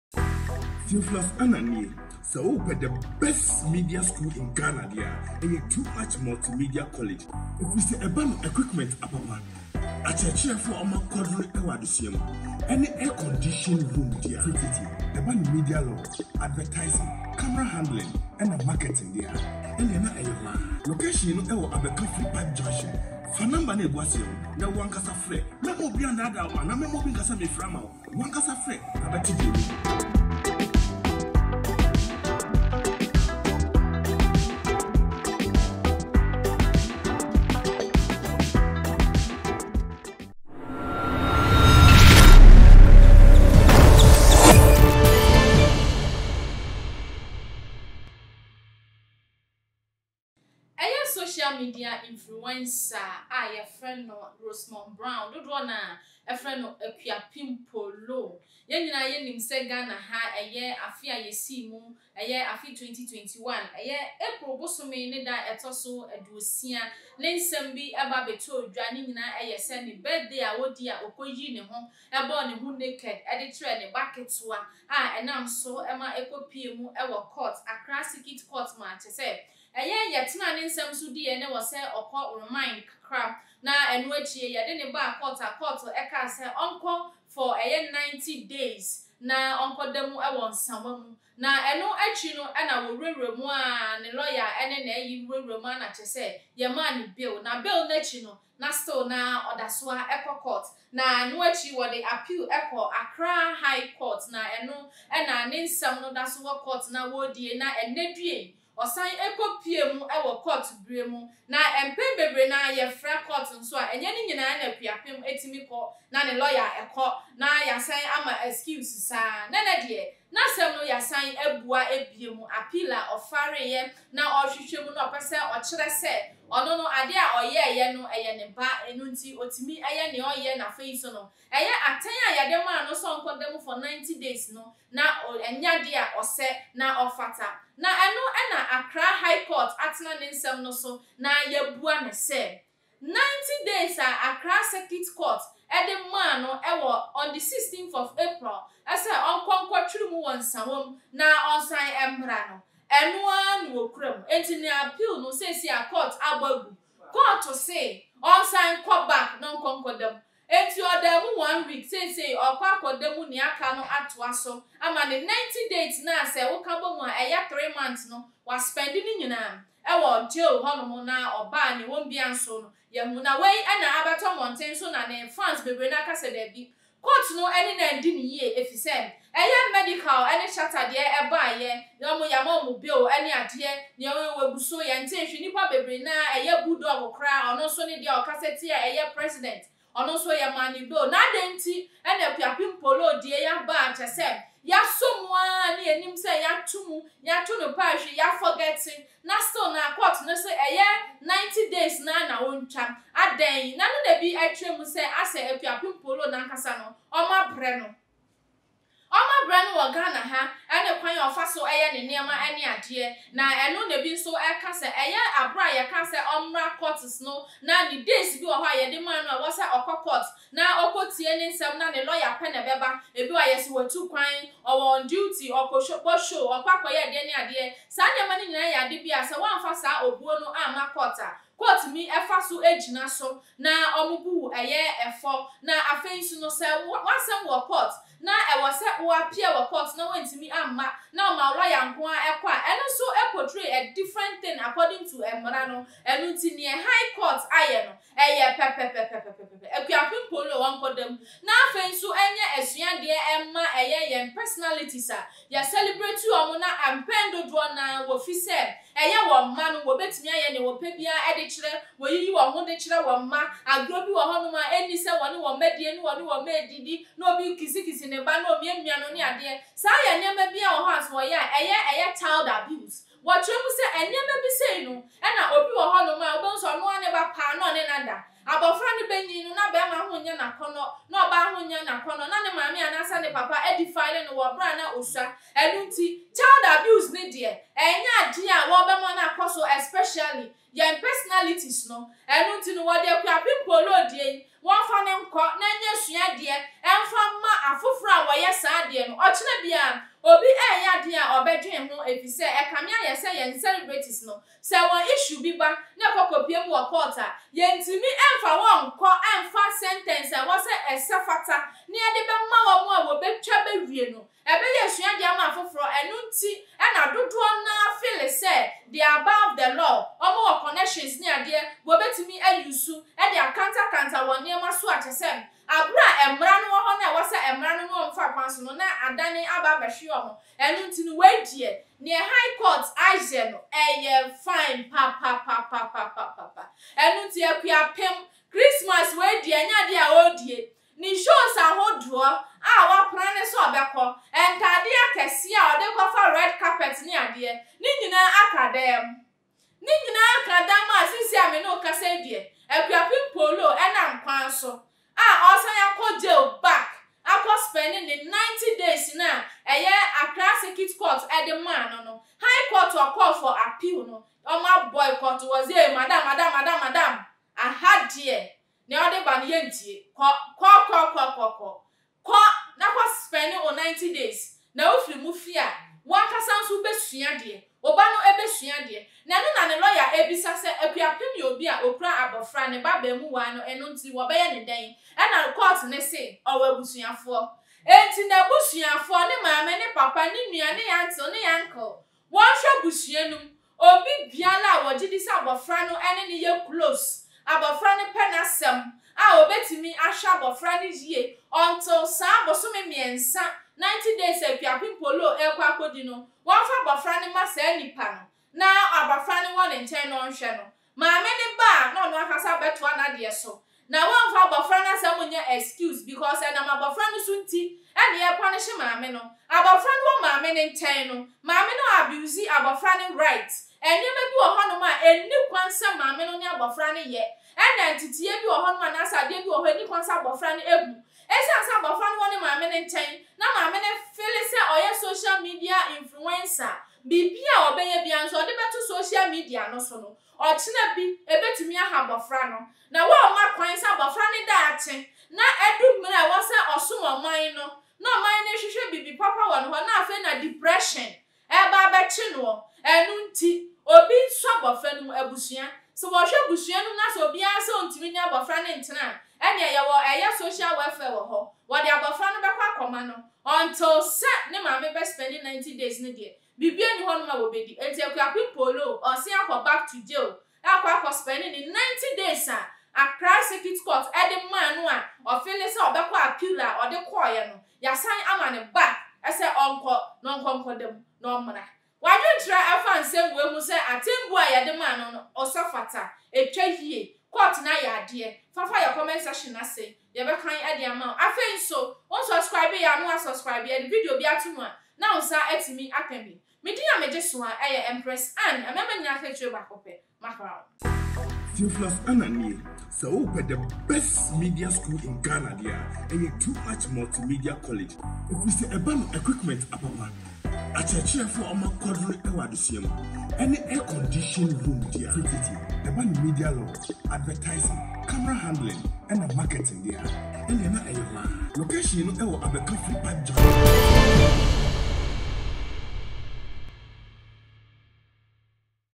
Break In; Akuapem Poloo Jailed For 90 days in Prison..
Adom Page is a channel that provides you latest entertainment and hottest trends in Africa.